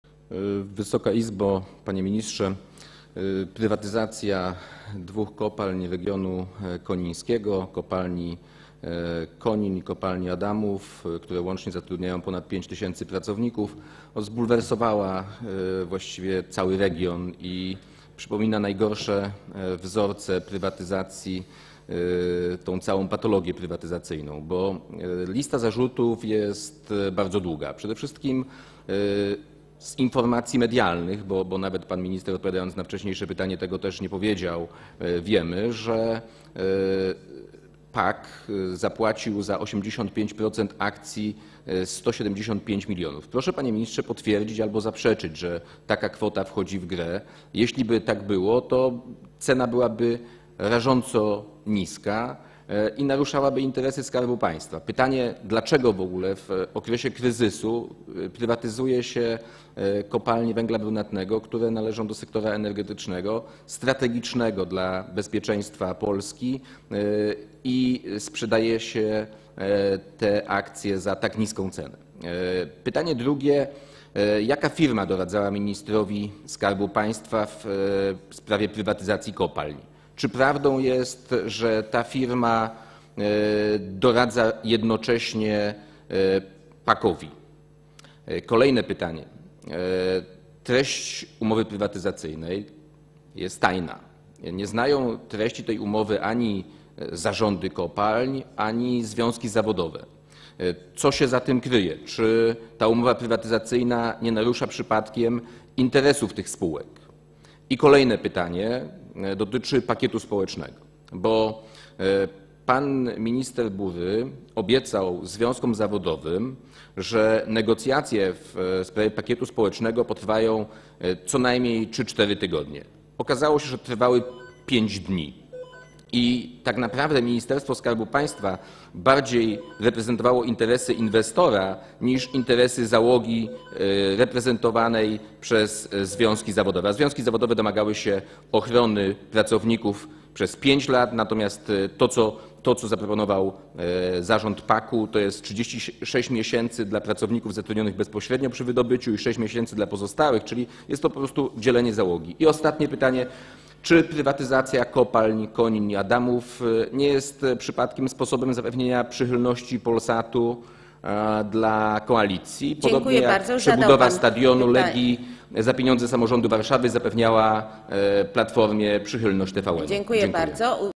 [78/358] Zbigniew Dolata: Wysoka Izbo! Panie Ministrze! Prywatyzacja dwóch kopalń regionu koniń..
Posiedzenie Sejmu nr 16
14 czerwca 2012 r. 10:35:54
Poseł Zbigniew Dolata:

Wysoka Izbo!  Panie Ministrze!  Prywatyzacja dwóch kopalń regionu konińskiego, kopalni Konin i kopalni Adamów, które łącznie zatrudniają ponad 5 tys.  pracowników, zbulwersowała właściwie cały region, a przypomina najgorsze wzorce prywatyzacji, tą całą patologię prywatyzacyjną.  Lista zarzutów jest bardzo długa.

Przede wszystkim z informacji medialnych, bo pan minister, nawet odpowiadając na wcześniejsze pytanie, tego nie powiedział, wynika, że PAK zapłacił za 85% akcji 175 mln.  Proszę, panie ministrze, potwierdzić albo zaprzeczyć, że taka kwota wchodzi w grę.  Jeśliby tak było, to cena byłaby rażąco niska i naruszałoby to interesy Skarbu Państwa.  Pytanie: Dlaczego w ogóle w okresie kryzysu prywatyzuje się kopalnie węgla brunatnego, które należą do sektora energetycznego, strategicznego dla bezpieczeństwa Polski, i sprzedaje się ich akcje za tak niską cenę?  Pytanie drugie: Jaka ﬁrma doradzała ministrowi skarbu państwa w sprawie prywatyzacji kopalń?

Czy prawdą jest, że ta ﬁrma doradza jednocześnie PAK?  Następne pytanie.  Treść umowy prywatyzacyjnej jest tajna.  Nie znają treści tej umowy ani zarządy kopalń, ani związki zawodowe.  Co się za tym kryje?

Czy ta umowa prywatyzacyjna nie narusza przypadkiem interesów tych spółek?  Kolejne pytanie dotyczy pakietu społecznego.  Pan minister Bury obiecał związkom zawodowym, że negocjacje w sprawie pakietu społecznego potrwają co najmniej 3, 4 tygodnie.

(Dzwonek)

Okazało się, że trwały 5 dni i tak naprawdę Ministerstwo Skarbu Państwa w większej mierze reprezentowało interesy inwestora niż interesy załogi reprezentowanej przez związki zawodowe.  Związki zawodowe domagały się ochrony pracowników przez 5 lat, natomiast to, co zaproponował Zarząd PAK, to 36 miesięcy dla pracowników zatrudnionych bezpośrednio przy wydobyciu i 6 miesięcy dla pozostałych, czyli jest to po prostu dzielenie załogi.  I ostatnie pytanie: Czy prywatyzacja kopalń Konin i Adamów nie jest przypadkiem sposobem zapewnienia przychylności Polsatu dla koalicji...

Wicemarszałek Wanda Nowicka: Dziękuję bardzo. Już zadał pan pytania.

...podobnie jak przebudowa stadionu Legii za pieniądze samorządu Warszawy, która zapewniała Platformie przychylność TVN?  Dziękuję.